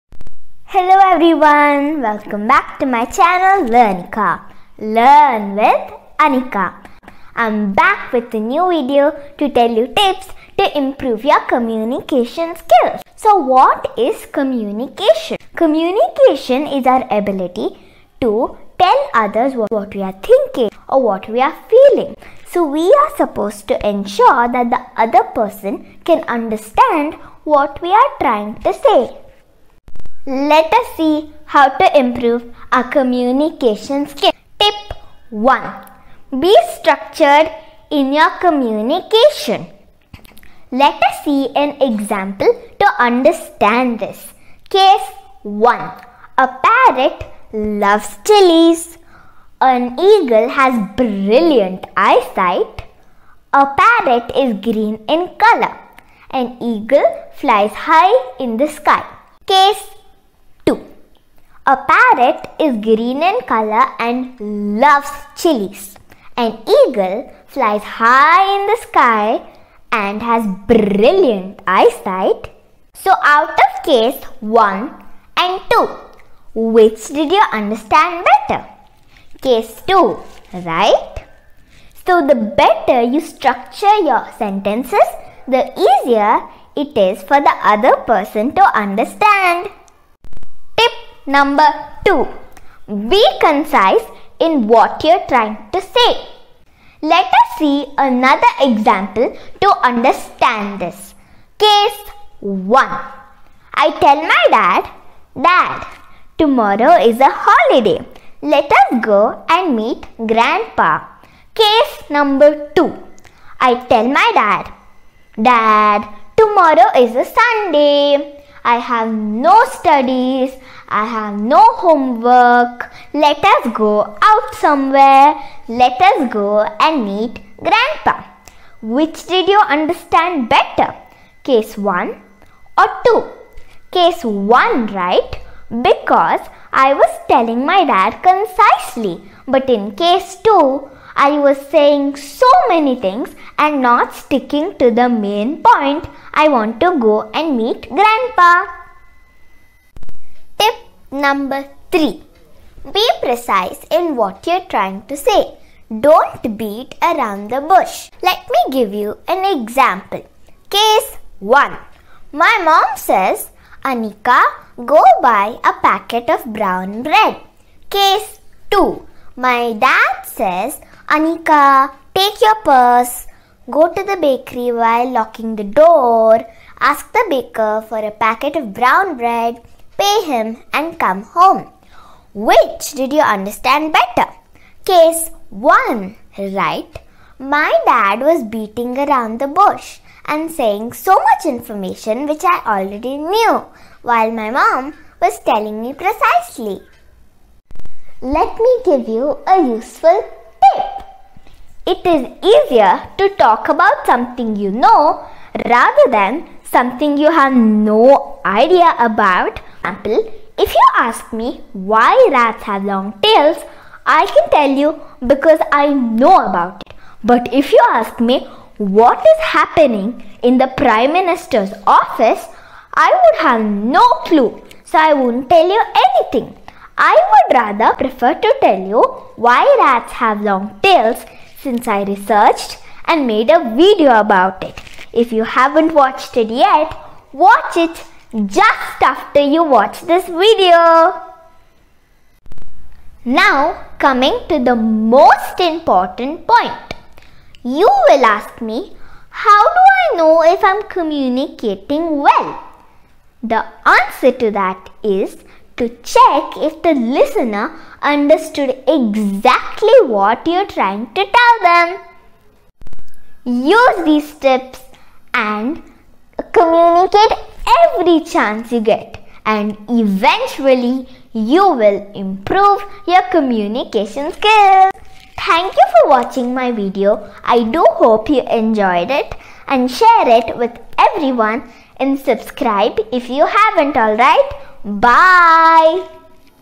Improve your communication skills in 3 simple steps
Being able to communicate effectively is one of the most important life skill.
Communication is needed in all walks of life. Remember a good communicator is considered a "people person"!
 Through this video, Anika helps you improve your communication skills in just 3 simple steps.